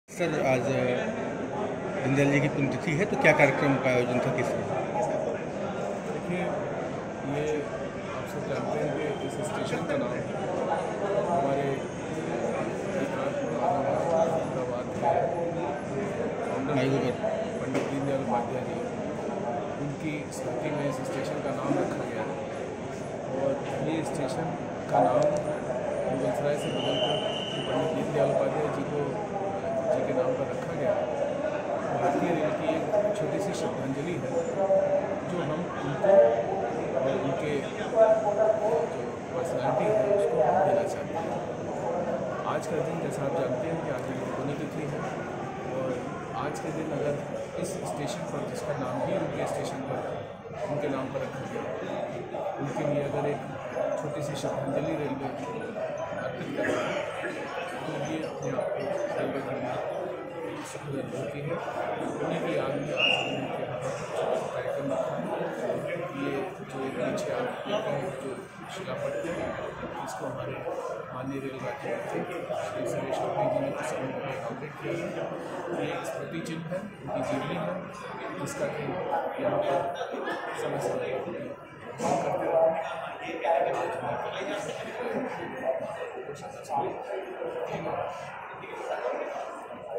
DRM ने दीन दयाल उपाध्याय को दी श्रद्धांजलि
पंडित दीनदयाल उपाध्याय जी के पुण्यतिथि पर पंडित दीनदयाल नगर दीनदयाल उपाध्याय मंडल के डीआरएम ने उनकी तैल चित्र पर माल्यार्पण कर उनकी पुण्यतिथि मनाई।